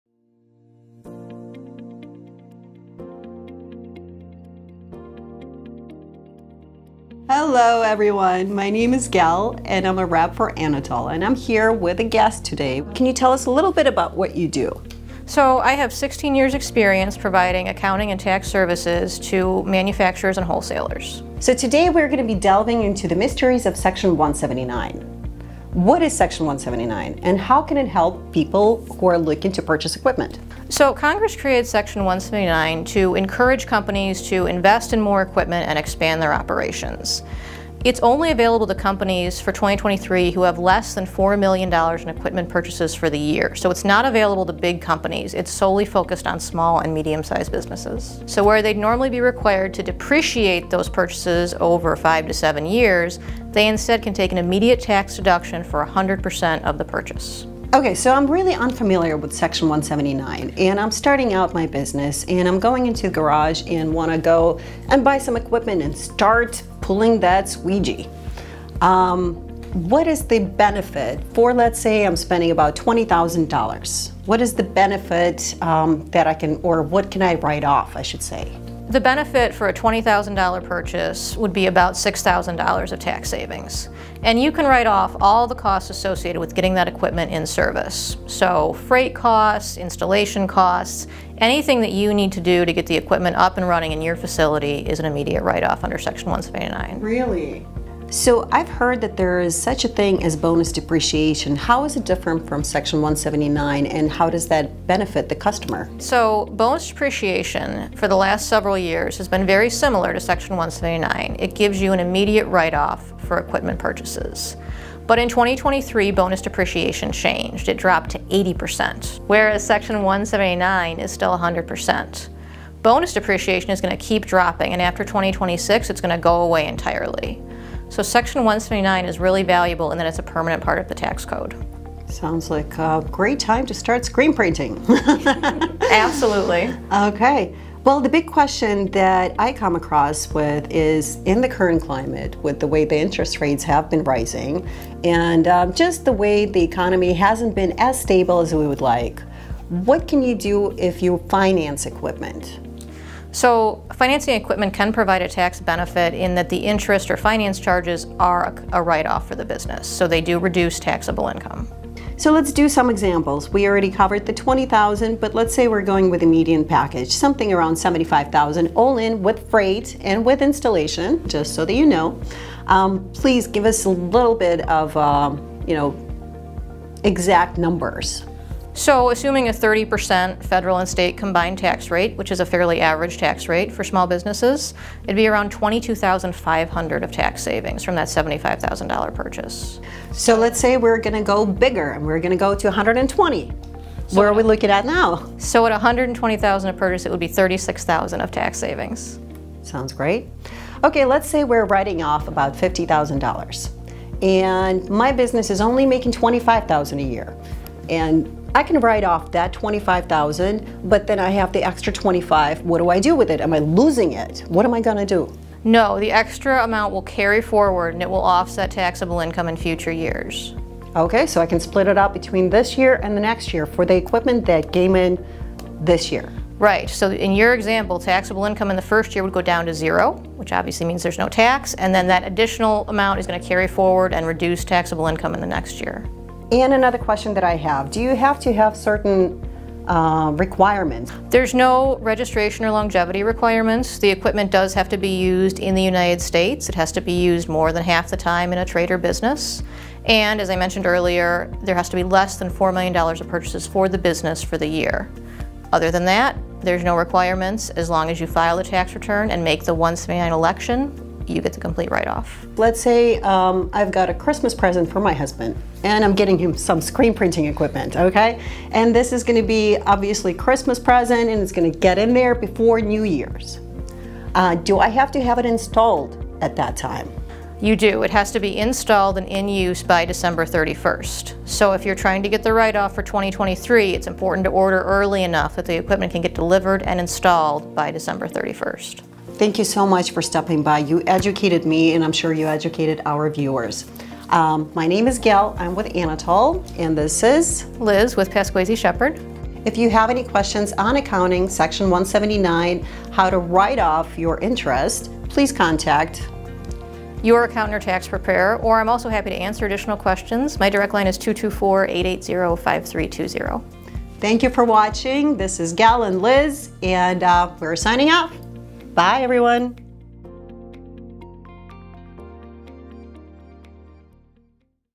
Screen Printers Can Save Thousands with Section 179!
Need to upgrade your screen printing shop with new equipment and want to save big at tax time? Anatol Regional Sales Manager Gelena Dearwood and Liz Slowinski, CPA from Pasquesi Sheppard LLC discuss how screen printers who purchase equipment can save thousands with Section 179.